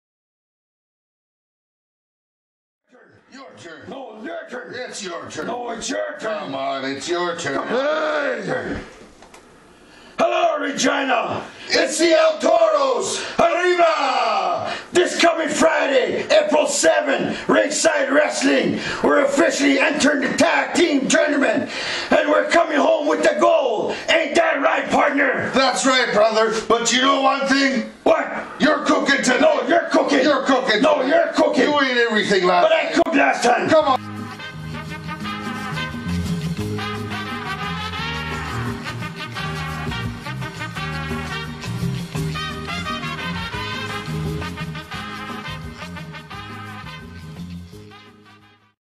El Toro's Ringside Wrestling promo
El Toro 1 & 2 officially enter the Ringside Wrestling tag team tournament searching for tag team gold this coming Friday April 7/17